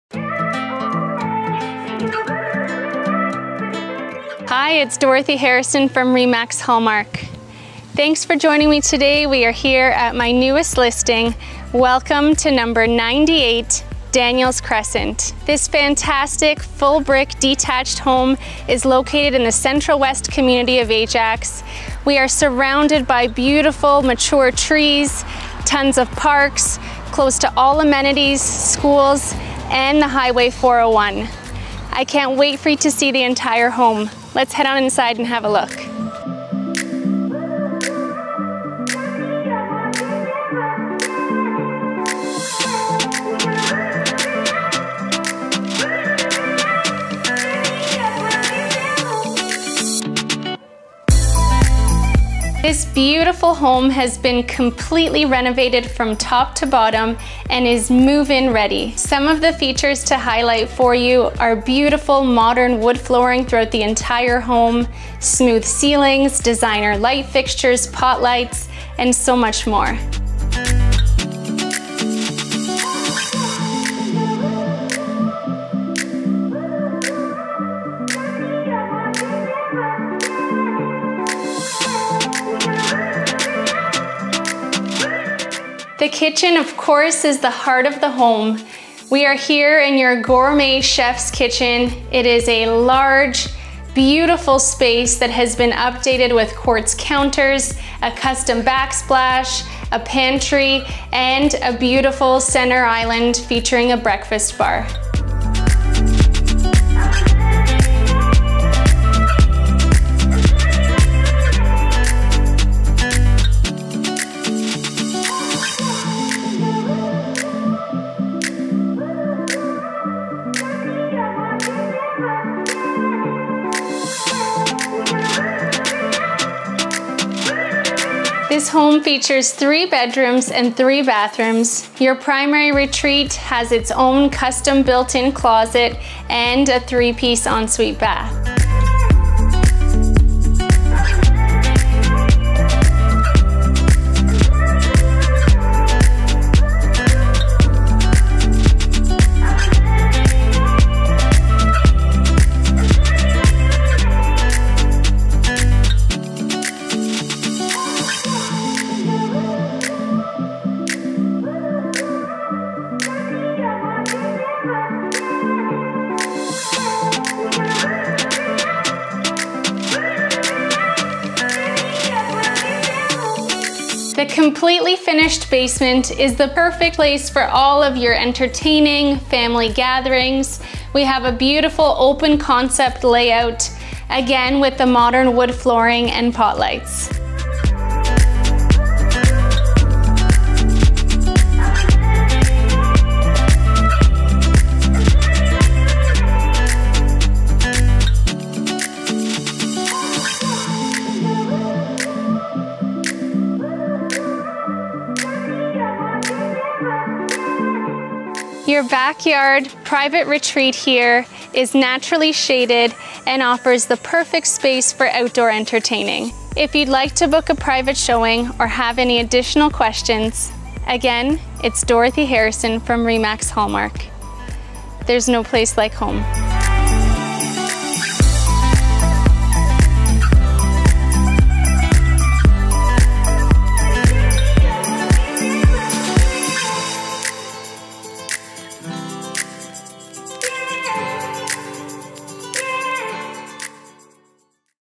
98 Daniels Crescent, Ajax Real Estate Sold By Dorothy
Welcome to 98 Daniels Cres. Ajax real estate Located In The Mature Central West Community Of Ajax.

This impeccably renovated home exudes the charm and elegance that could grace the pages of a high-end interior design magazine. Every detail of this residence has been thoughtfully upgraded, resulting in a harmonious blend of modern luxury and functional comfort.

Upon entering the main floor, you're greeted by the warmth of wide plank modern wood flooring that spans the entirety of the space. The ceilings have been rendered smooth, creating a sleek and contemporary atmosphere, further enhanced by the gentle glow of potlights that artfully illuminate the rooms.

The heart of this home lies in the gourmet kitchen—a true culinary haven. Adorned with quartz countertops that gleam under the light, the kitchen exudes both beauty and functionality. Custom cabinets, featuring undermount lighting, provide ample storage while exuding a sense of refinement. The tasteful backsplash adds a touch of personality, while a well-appointed pantry ensures that all your essentials are neatly organized. The kitchen's allure is completed by a convenient walkout to a spacious yard, allowing for seamless indoor-outdoor living.

As you ascend the oak staircase to the second floor, you'll discover a haven of relaxation in the form of three bedrooms. The primary bedroom is a sanctuary in itself, boasting a custom-built closet that not only adds a sense of luxury but also keeps everything organized. The attached 3-piece ensuite bathroom is a private oasis, complete with a view overlooking the serene backyard.

The lower level of this home has also been transformed into a versatile space. Finished to perfection, it offers an ideal area for entertaining guests or creating a cozy retreat. This level could even serve as a potential fourth bedroom or guest suite, complete with an adjacent office space that could cater to a variety of needs.

Situated on a street safe for children, this home is a fantastic option for first-time buyers. The double-wide driveway, conveniently devoid of a sidewalk, accommodates up to four cars—an advantageous feature in itself. Proximity to a range of amenities, including schools, parks, shopping, and easy access to Highway 401, further enhances the allure of this property.

In summary, this home is a true gem, embodying the essence of a magazine-worthy living space. Its stunning renovations, from top to bottom, promise a harmonious blend of style, functionality, and comfort—a place where you can create lasting memories and enjoy a lifestyle tailored to modern sensibilities.

For More Details On This Property Or
"There No Place Like Home!"

Soldbydorothy.com

Ajax Real Estate Sold By Dorothy